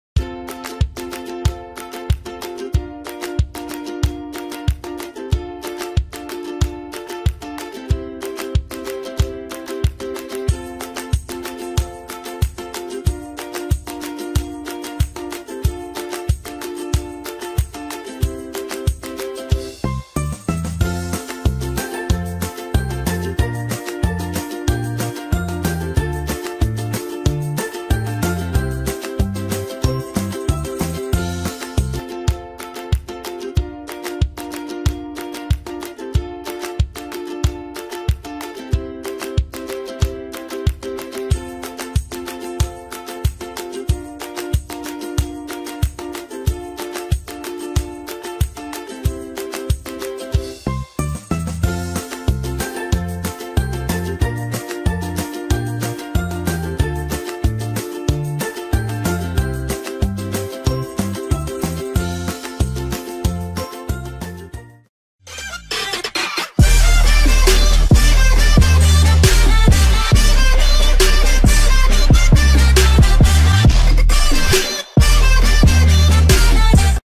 DVD Drive Missing windows 10 Eazy Solved
How to Fix DVD Drive Missing From File Explorer in Windows 7/8/10